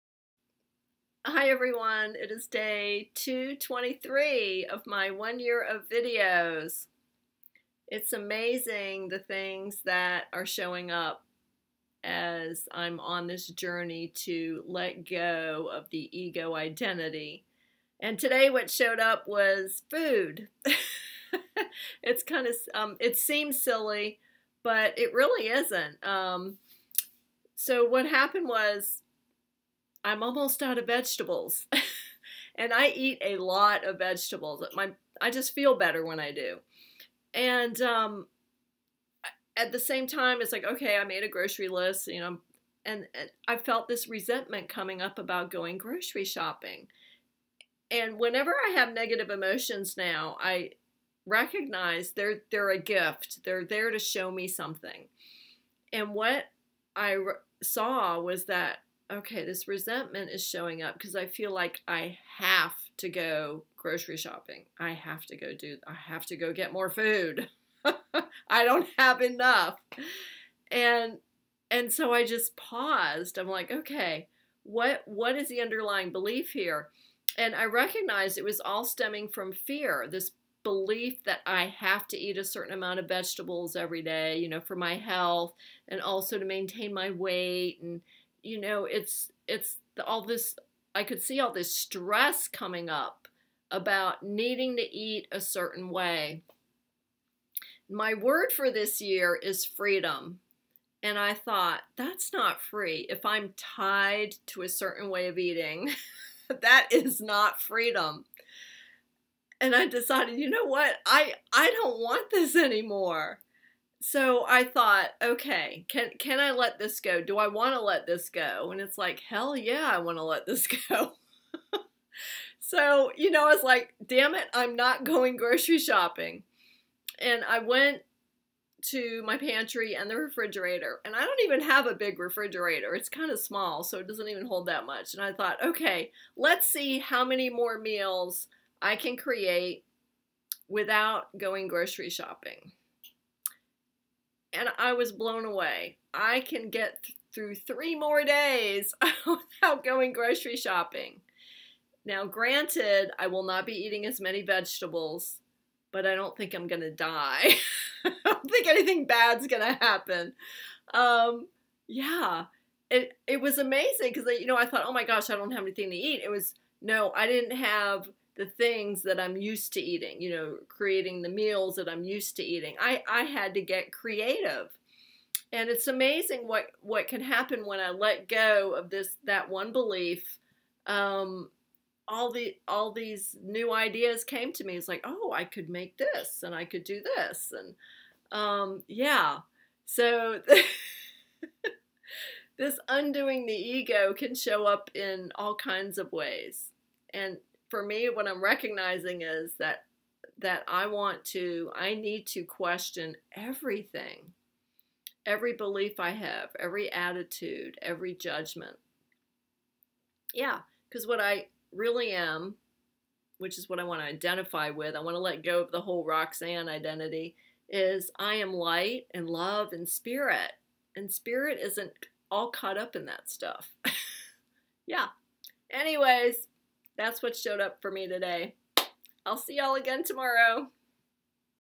Day 223 - Questioning beliefs
My word for this year is FREEDOM. That word is making me aware of things in my life that limit my freedom - beliefs, attitudes, behaviors - anything that has me identified with the ego. It is my desire to be free of the ego and to identify with the truth of who I am - love, light, spirit.

A very specific subject showed up today that I wanted to look at - food. I am currently out of vegetables. I make a point of eating lots of them. Nothing wrong with that. I noticed though that the thoughts in my head were insistent that I needed to go to the grocery store to get more, along with a list of other things. I recognized that I was tied to a certain way of eating. That word “tied” is a clue that I am not free because I am identifying with a certain belief - that I need to have large quantities of veggies every day. Lots of fear shows up around food for me. My desire for freedom is becoming greater than that fear. Resentment shows up as a clue to warn me when I am giving in to an old paradigm that doesn’t work for me any more.

I actually felt resentment show up about going grocery shopping because it felt like I HAD to go (that was the ego making demands). I decided to question that demand. I had the thought, “Do I want to let go of this belief that I need to eat lots of vegetables every day?” The answer was, “Yes!” The next question was, “I wonder how many more meals I can create from what I have in the pantry and refrigerator?” I did an inventory. It’s amazing how creative the mind can get when it is faced with a challenge. When I was caught up in fear and the belief that I needed to eat a lot of vegetables, it didn’t look like I had much left to make some decent meals. I was pleasantly surprised to discover that I actually have enough food to get through Wednesday - that’s three more days! As silly as this whole example may seem, in undoing the ego I am willing to question EVERYTHING and bring it to the truth of who I am.

When I let go of the belief that things need to be a certain way in order for me to be ok, I open the door to limitless possibilities and to the peace and freedom that are waiting for me.